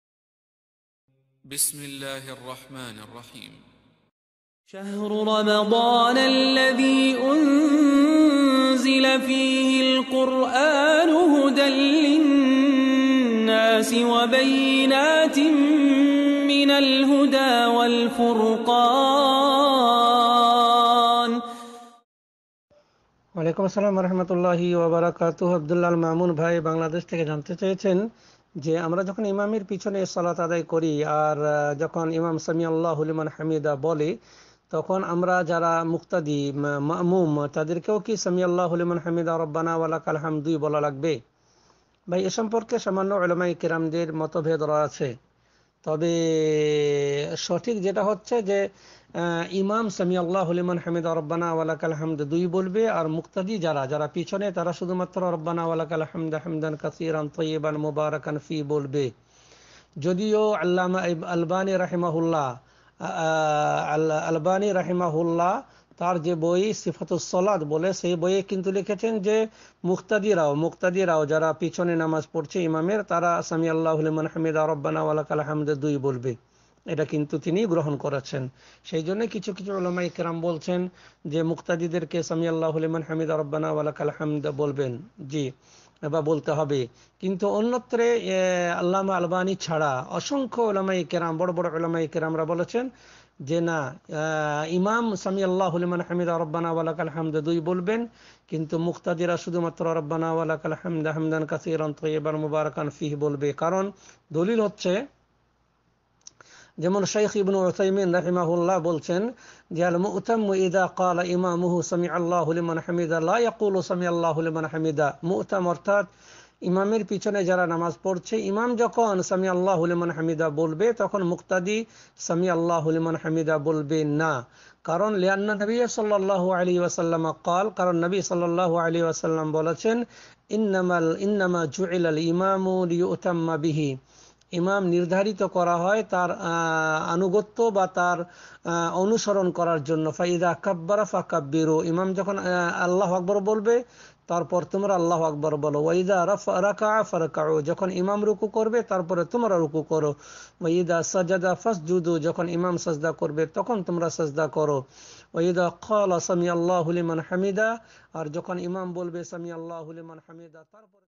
ইমাম যখন সামি আল্লাহ হুলিমান হামিদা বলে আমরাও কি সে কথাগুলো বলতে পারি? আলোচক শফিউর রহমান।
ইমাম যখন সামি  আল্লাহ হুলিমান হামিদা বলে আমরাও কি সে কথাগুলো বলতে পারি? আলোচক শফিউর রহমান। স্বাগতম আপনাকে AL FURQAN media.